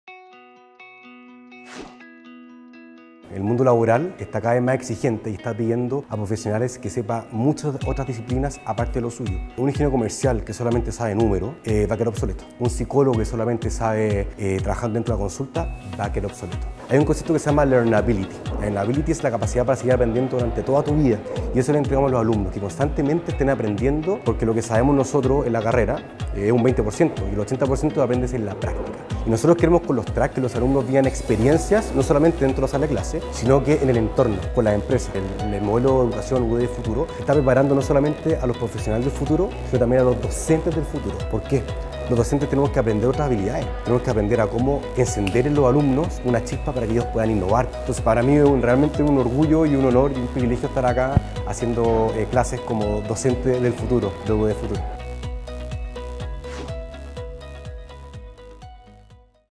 Experiencias UDD Futuro - Track Ciencia, Tecnología e Innovación / Santiago
"Un ingeniero comercial que solamente sabe de números, va a quedar obsoleto. Un psicólogo que solamente sabe trabajar dentro de una consulta va a quedar obsoleto", afirma Ricardo González, profesor de Design Sprint y Prototipado Digital, del track Ciencia Tecnología e Innovación, acerca del desafiante mundo laboral, que está pidiendo profesionales más integrales. Esto en el marco del primer año de implementación del proyecto educativo UDDFuturo.